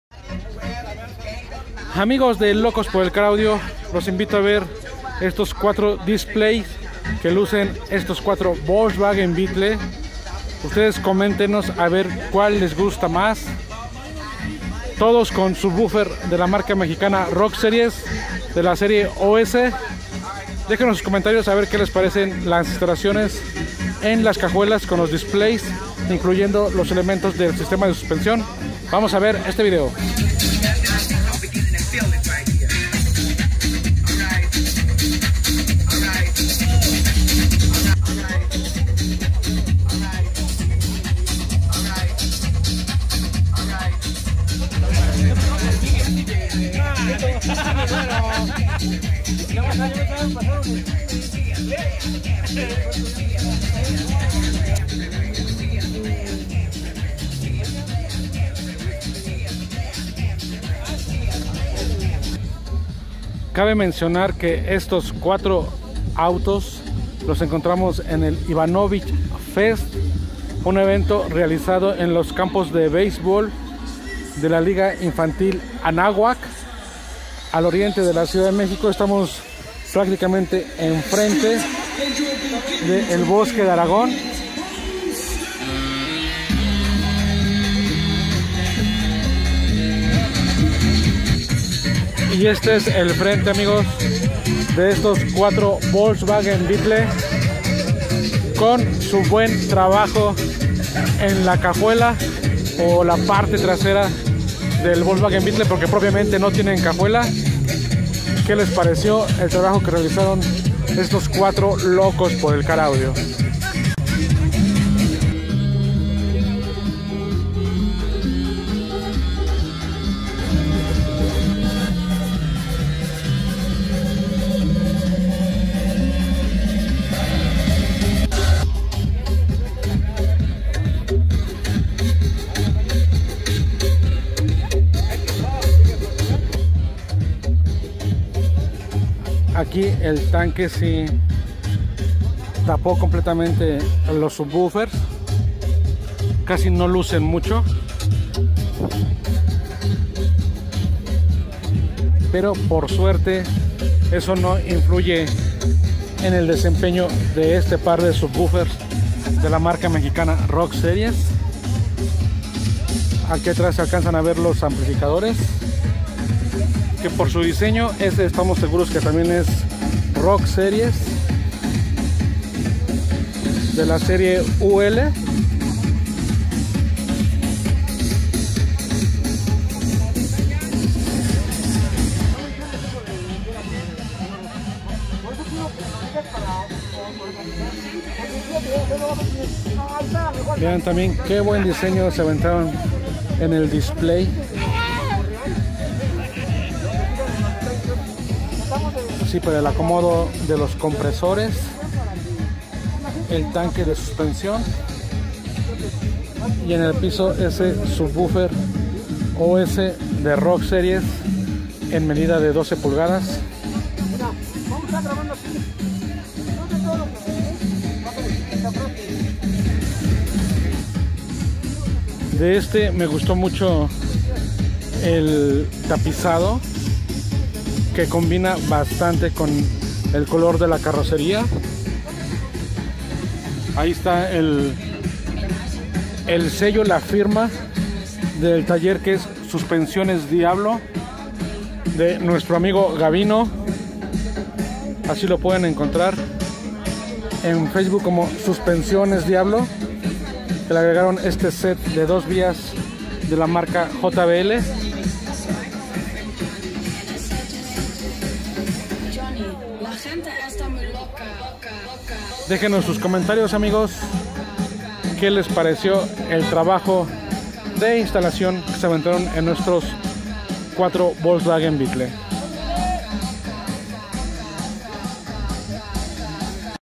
¡4 DISEÑOS en CAJUELA de Beetle! ¿Cuál les gusta más? Todos con SUBWOOFERS Rock Series.
Estuvimos presentes en el Ivanovich Fest y nos encontramos con estos 4 Volkswagen Beetle portando interesantes diseños en el display de la cajuela, llama la atención que a todos les guste el desempeño de los subwoofers Rock Series.
¿A ustdes cuál les gusta más?